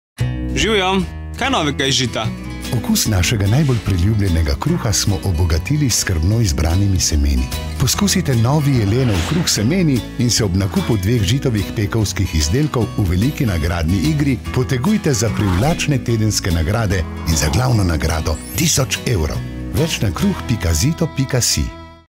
Žito kruh - Jelen s semeni
Novo v družini Jelenov - Jelen s semeni in velika nagradna igra, ki prinaša bogate nagrade.